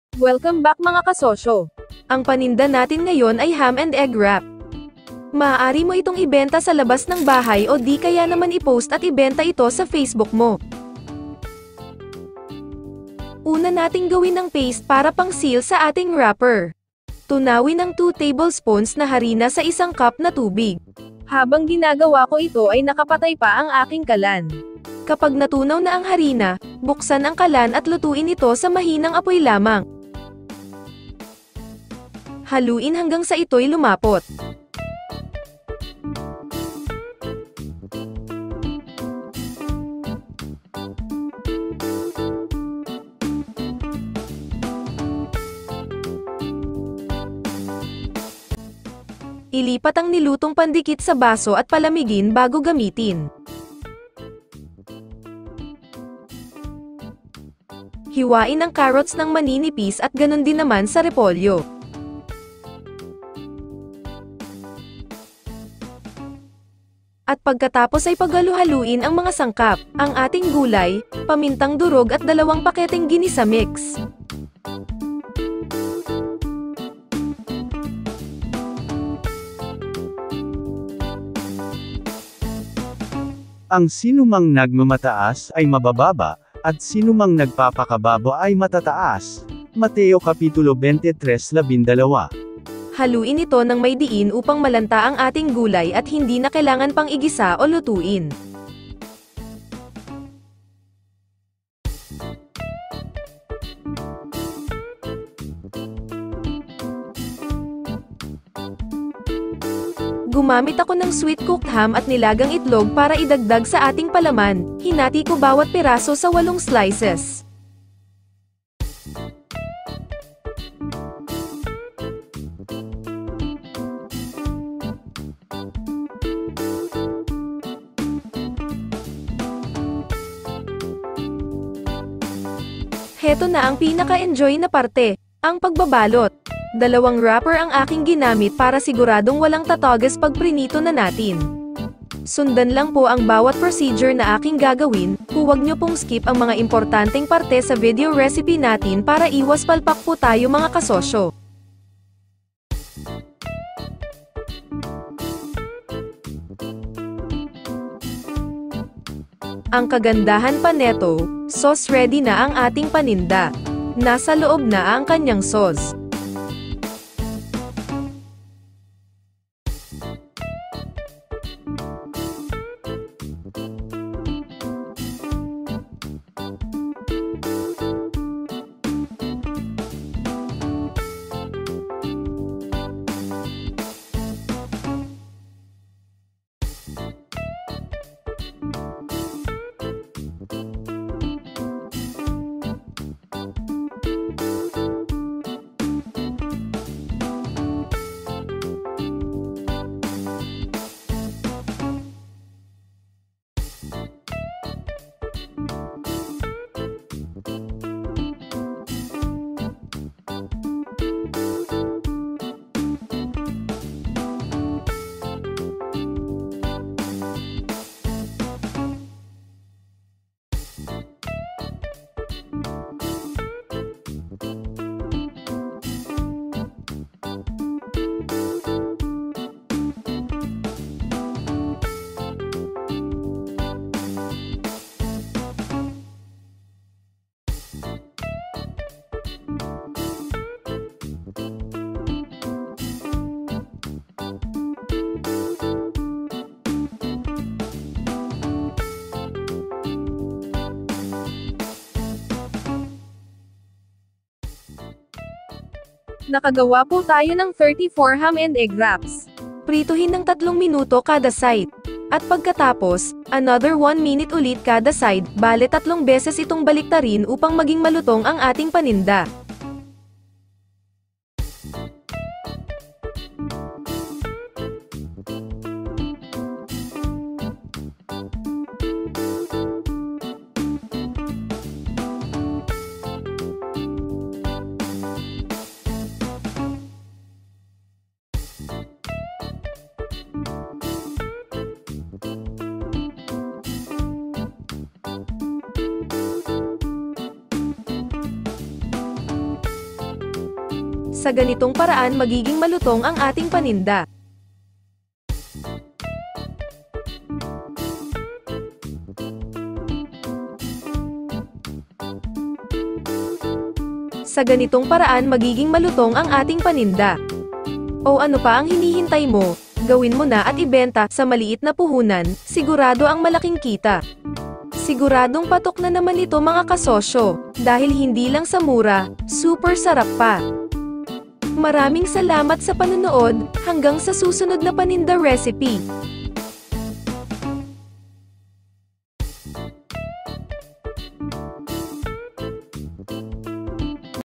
Pinoy ham and egg wrap❗️Patok pang negosyo❗️streetfood recipe
Ingredients:
1kilo repolyo
1/4kilo carrots
1tsp ground pepper
2 (8g/pack)ginisa mix
5boiled eggs
5 cooked ham
Mafran all purpose dressing
68pcs wrapper

PASTE FOR SEALING:
1cup water
2tbsp flour

Magagawa:34pcs
Puhunan:P260(as of 05/08/23)
Bentahan:15pesos
Tubo/neto:250pesos

*pinoy samosa|triangle shanghai|fried siomai using lumpia wrapper